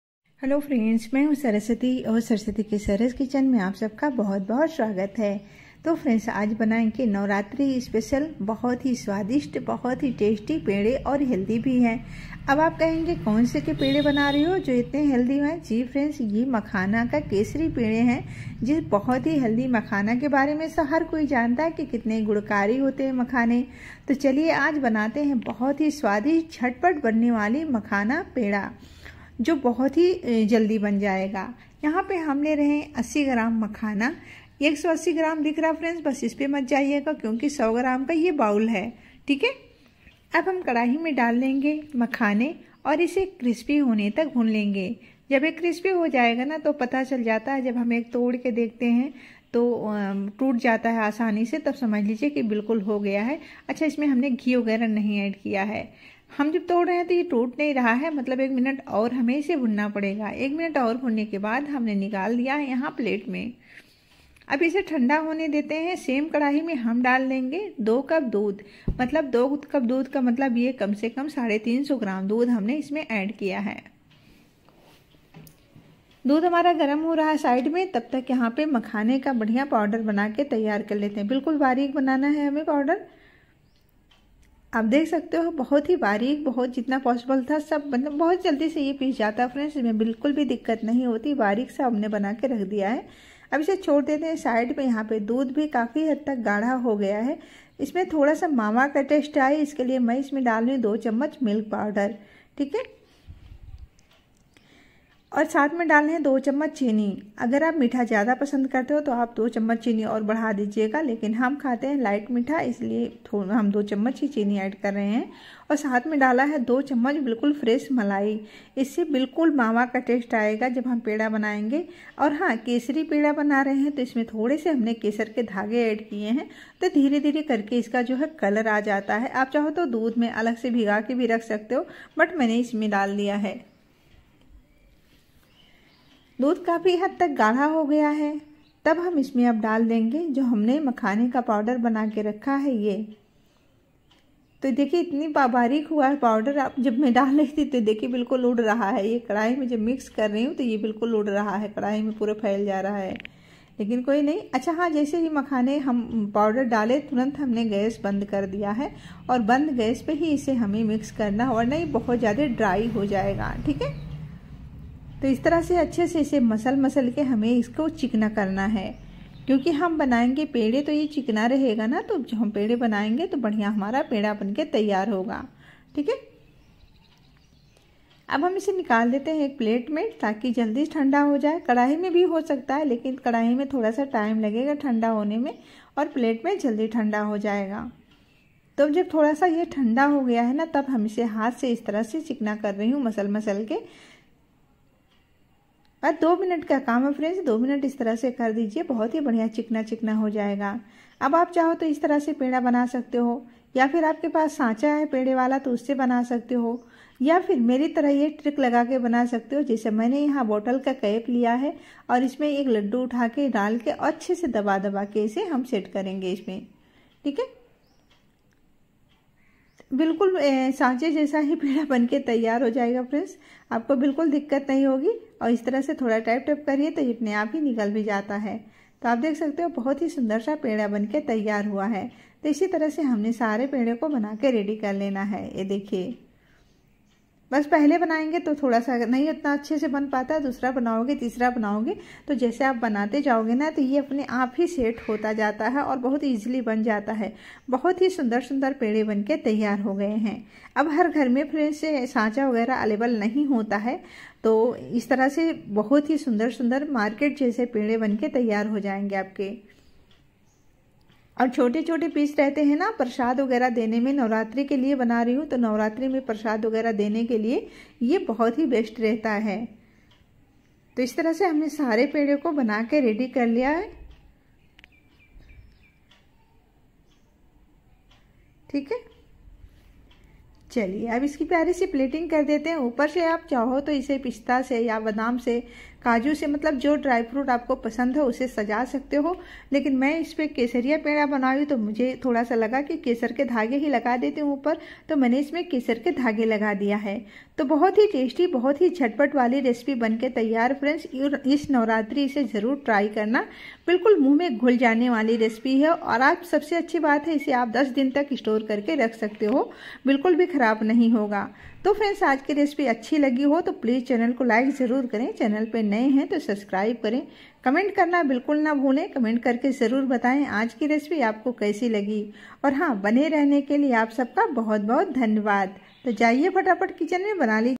नवरात्रि स्पेशल व्रत के लिए मखाना केसरिया पेड़ा || kesariya peda recipe || Saras kitchen
नवरात्रि स्पेशल मखाना
Kesariya peda Kesariya peda recipe